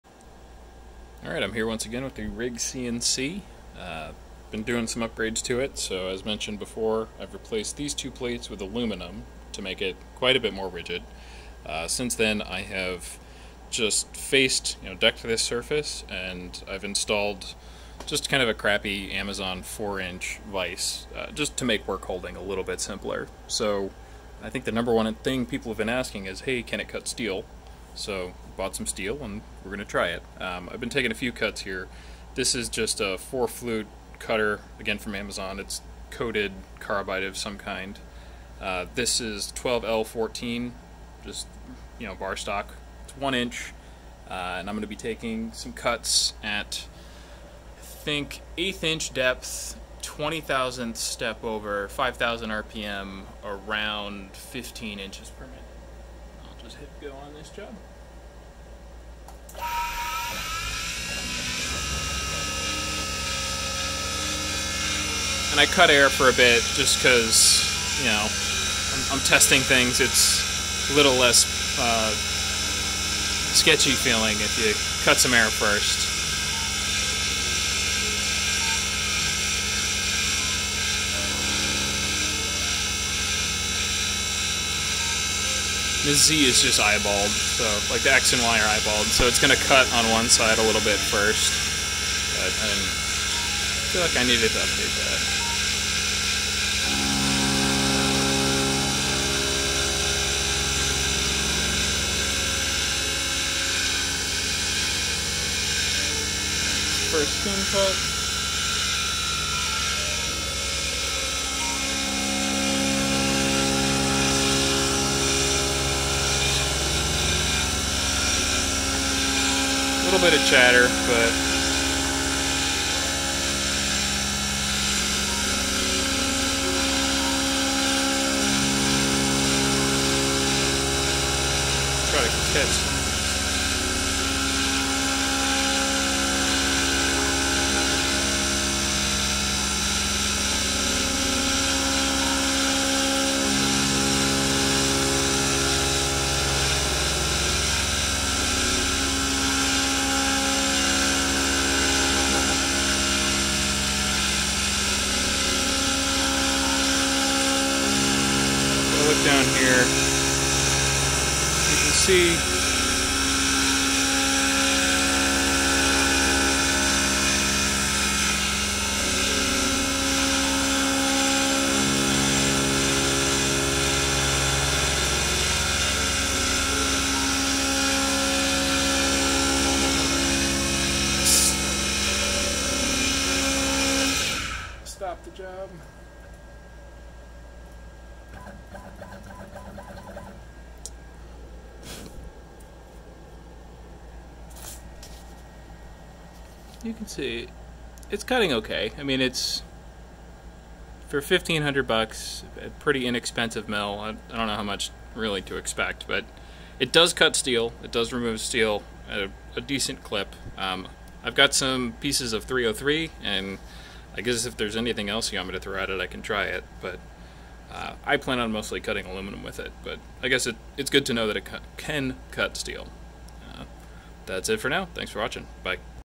RIGCNC-DIY VMC Cutting Steel
In this video I test cutting 12L14 steel with the RIGCNC. All materials purchased from Onlinemetals.com

More information on the mill can be found on both the website Rigcnc.com and on the playlists here on YouTube.